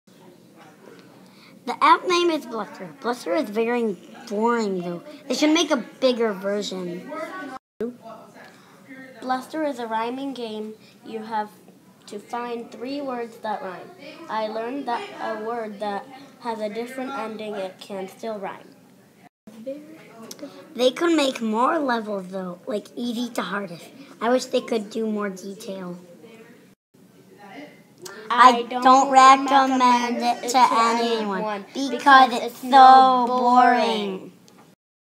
App Review: Bluster
Third Graders at Hillel Day School worked on their writing skills by reviewing iPad apps and creating these app reviews for you!